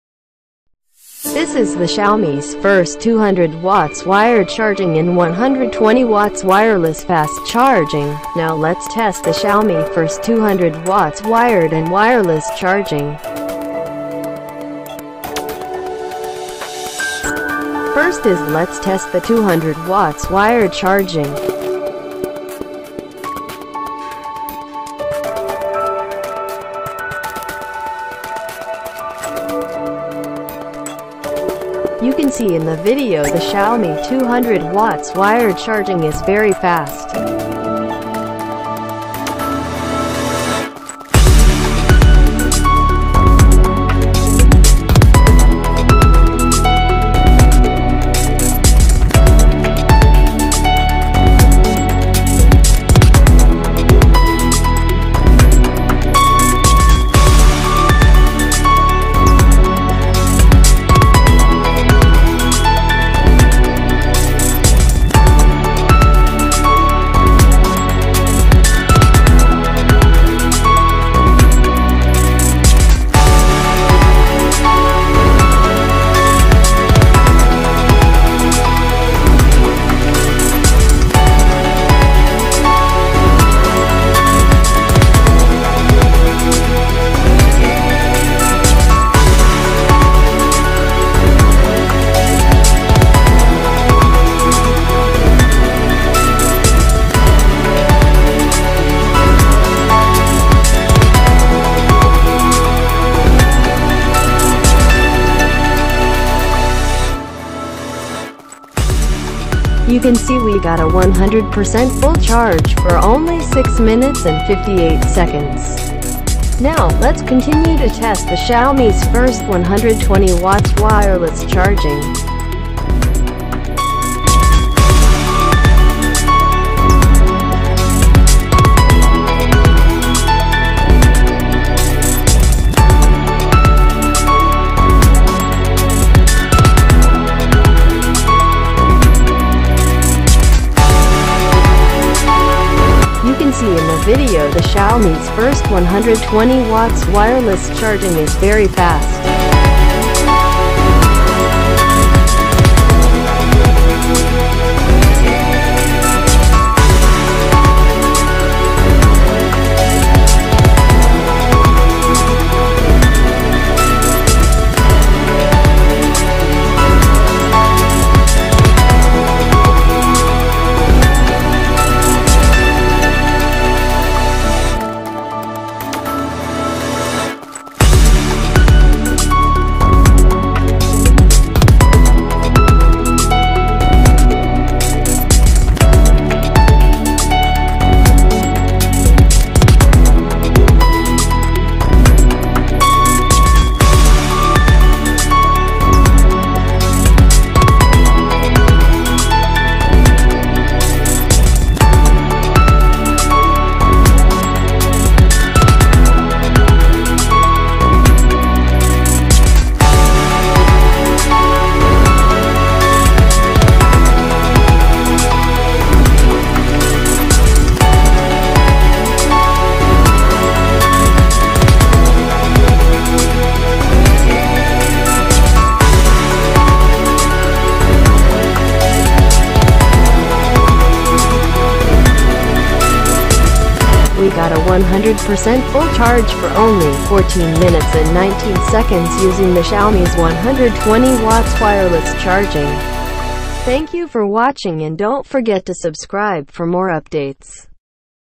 Xiaomi's First 200W Wired & 120W Wireless Fast Charging Real test
About:
The fast charging experience enters a new era today!

With industry-leading fast charging technology, you can charge a 4000mAh battery to 100% in just 8 minutes with our very first 200W wired charging! And it only takes 15 minutes using 120W wireless charging!